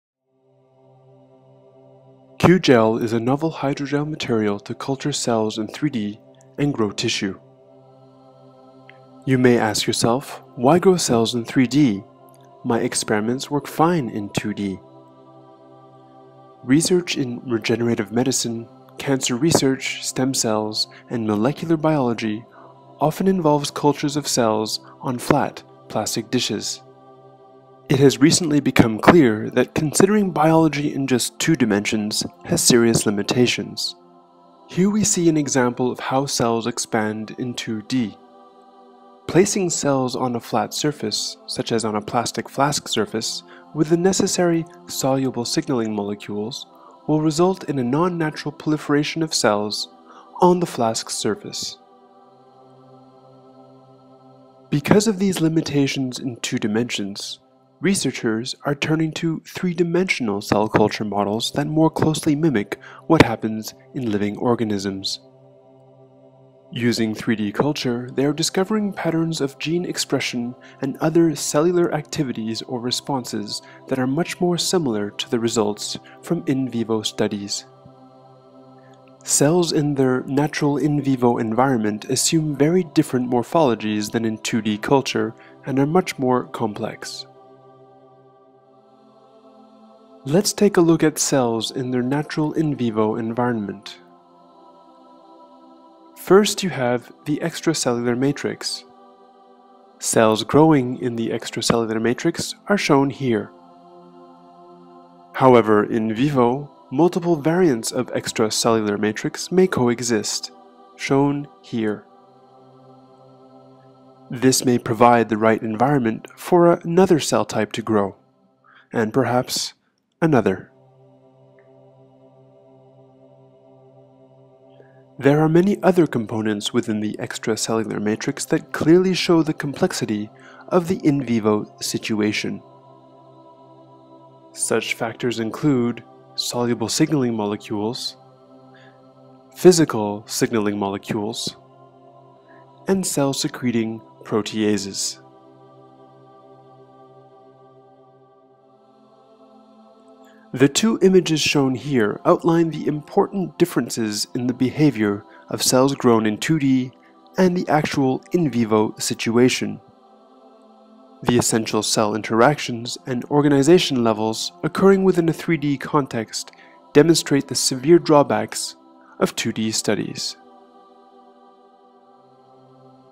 QGel: Why 3D Cell Culture?
A short video outlining 2D cell culture, and the limitations of 2D cell culture compared to the natural in vivo environment. More sophisticated models, such as 3D cell culture models, are necessary to more accurately represent in vivo conditions.